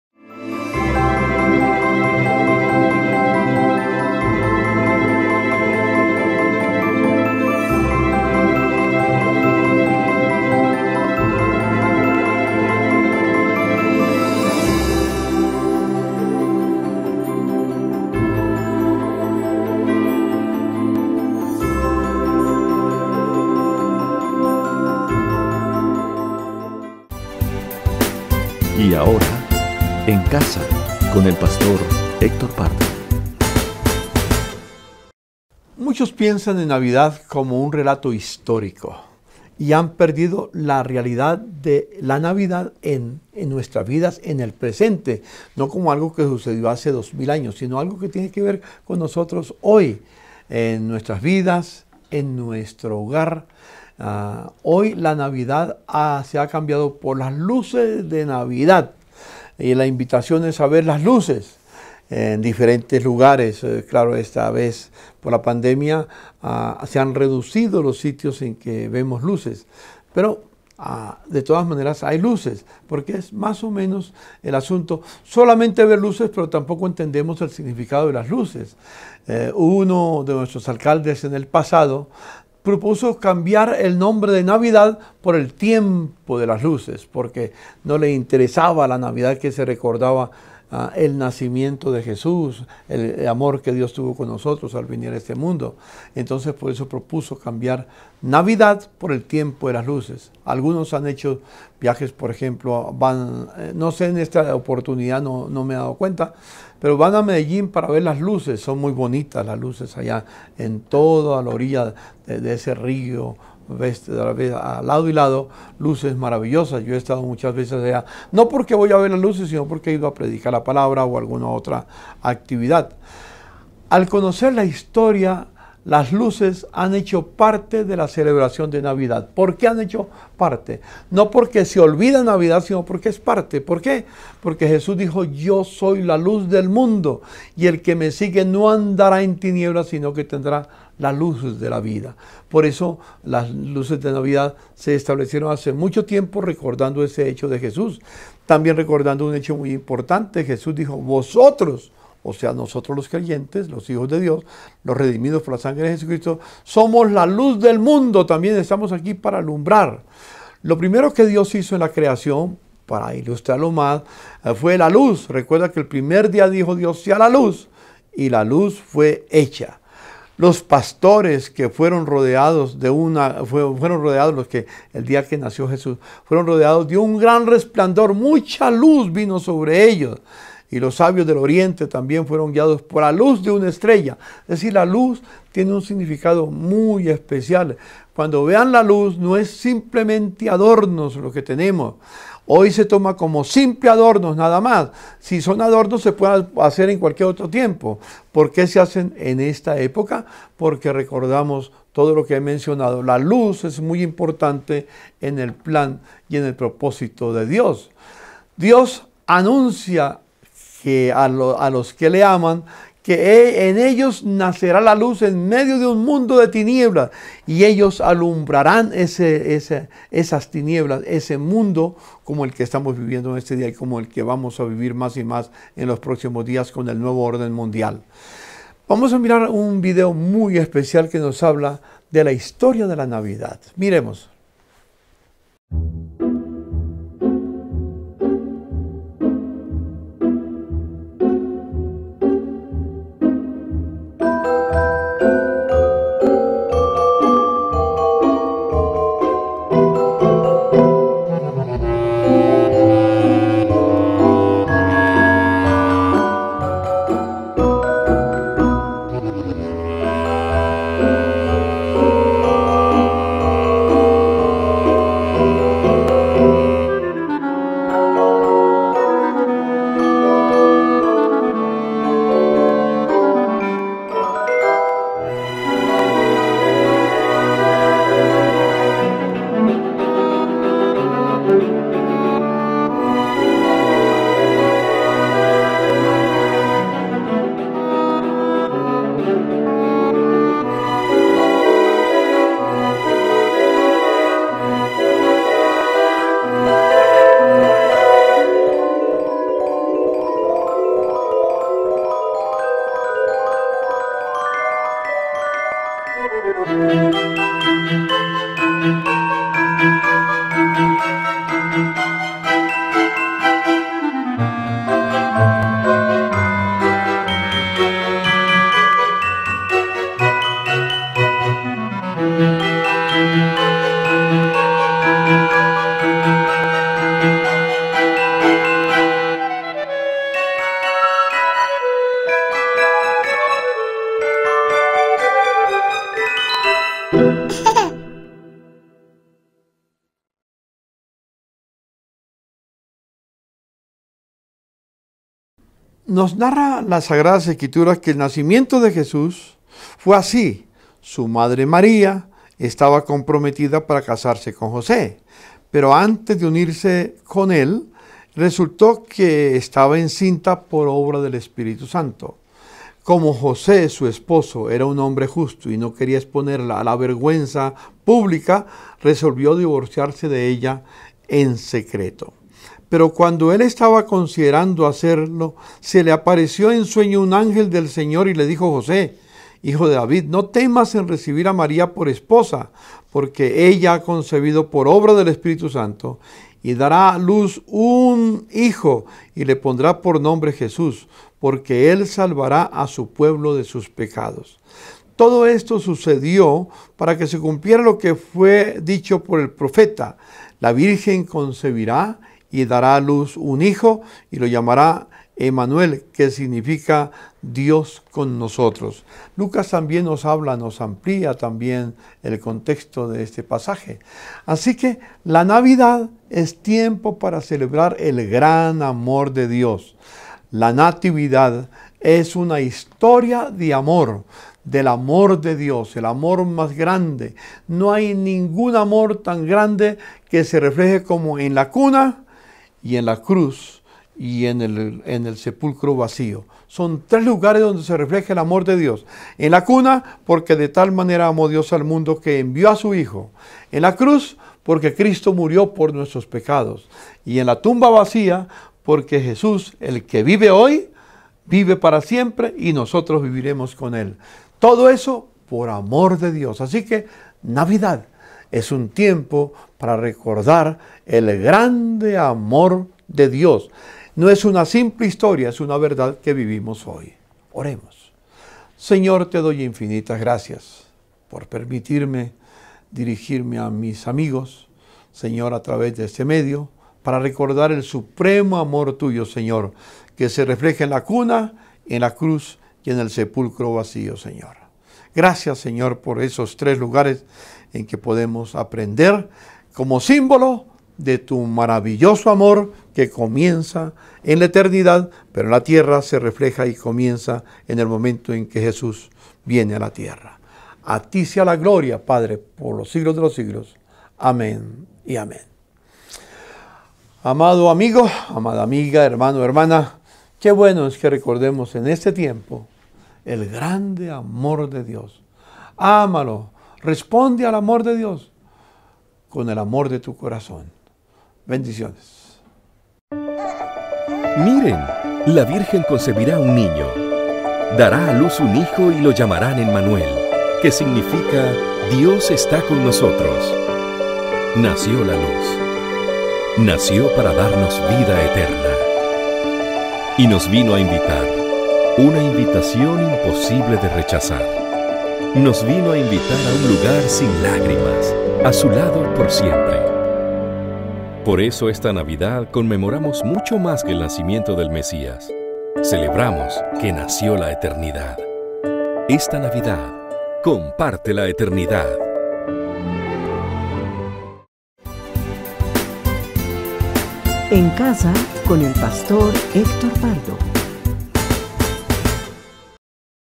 "HISTORIA INCOMPARABLE" EnCasaConHéctorPardo | 15 de Diciembre de 2020
De lunes a viernes a las 8:00 p.m. conéctate con el devocional EnCasaConHéctorPardo, un espacio de reflexión para compartir en familia y unirnos en oración.